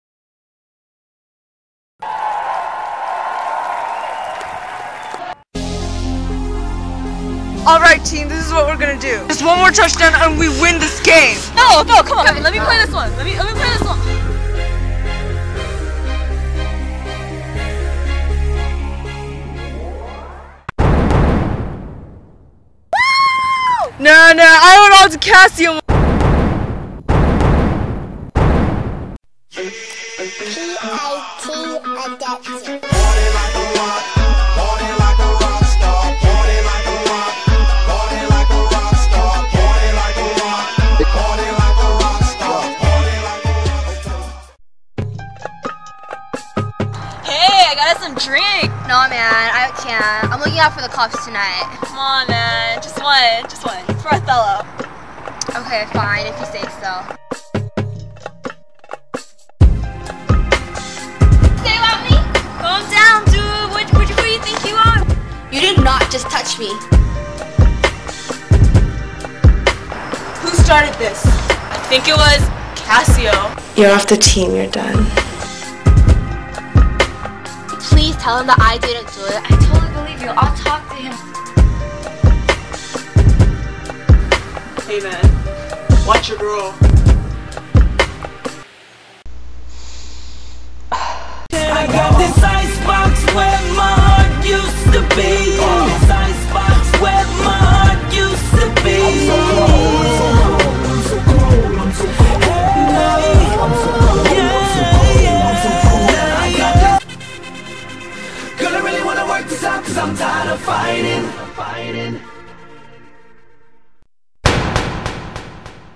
Desdemona Must Die - Trailer [Othello Commercial Project]
Desdemona Must Die Trailer
Cordero - English Honors
Othello Commerial Project

Julie Chung - Othello
Julia Namkoong - Cassio
Linda Luo - Desdemona
Melanie Yuen - Iago
Period 3

WE ARE NOT PROMOTING TEEN DRINKING.
TEEN DRINKING IS VERY BAD. KTHANKS =)